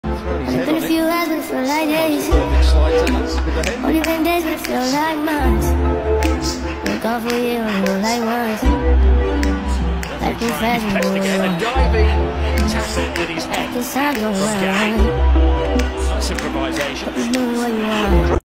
Gavi almost got his head kicked 🤯
Gavi head first slide tackle vs Dani Garcia during Athletic Bilbao vs FC Barcelona